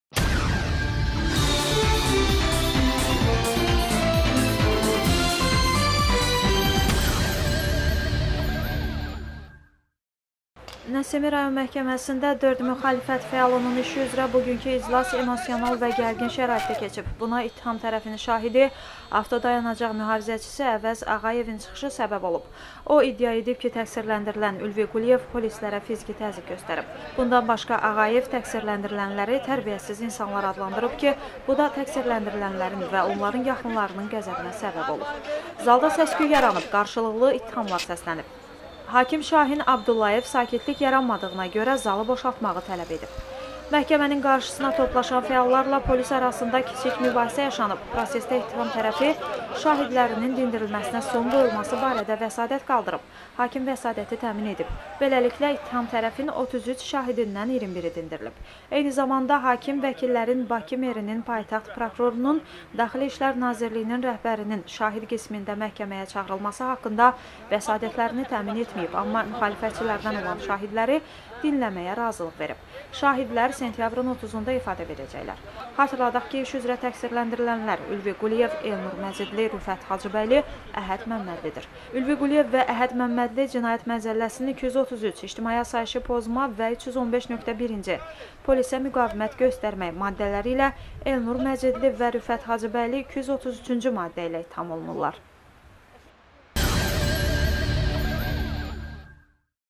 NƏSİMİ MƏHKƏMƏSİNDƏ QARŞILIQLI İTTİHAMLAR SƏSLƏNİB
Nəsimi rayon məhkəməsində dörd müxalifət fəalının işi üzrə bu günki iclas emosional və gərgin şəraitdə keçib. Buna ittiham tərəfin şahidi, avtodayanacaq mühafizəçisi Əvəz Ağayevin çıxışı səbəb olub.
    O iddia edib ki, təqsirləndirilən Ülvi Quliyev polislərə fiziki təzyiq göstərib. Bundan başqa, Ağayev təqsirləndirilənləri "tərbiyəsiz insanlar" adlandırıb ki, bu da təqsirləndirilənlərin və onların yaxınlarının qəzəbinə səbəb olub. Zalda səs-küy yaranıb, qarşılıqlı ittihamlar səslənib. Hakim Şahin Abdullayev sakitlik yaranmadığına görə zalı boşaltmağı tələb edib.  Məhkəmənin qarşısına toplaşan fəallarla polis arasında da kiçik mübahisə yaşanıb.

Prosesdə ittiham tərəfi şahidlərinin dindirilməsinə son qoyulması barədə vəsatət qaldırıb. Hakim vəsatəti təmin edib. Beləliklə, ittiham tərəfin 33 şahidindən 21-i dindirilib.
    Eyni zamanda, hakim vəkillərin Bakı merinin, paytaxt prokurorunun, DİN rəhbərinin şahid qismində məhkəməyə çağırılması haqqında vəsatətlərini təmin etməyib, amma müxalifətçilərdən olan şahidləri dinləməyə razılıq verib. Şahidlər sentyabrın 30-da ifadə verəcəklər.
    Xatırladaq ki, iş üzrə təqsirləndirilənlər Ülvi Quliyev, Elnur Məcidli,  Rüfət Hacıbəyli, Əhəd Məmmədlidir.
    Ülvi Quliyev və Əhəd Məmmədli CM-in 233 (ictimai asayişi pozma) və 315.1-ci (polisə müqavimət göstərmək) maddələri ilə, Elnur Məcidli və Rüfət Hacıbəyli 233-cü maddə ilə ittiham olunurlar.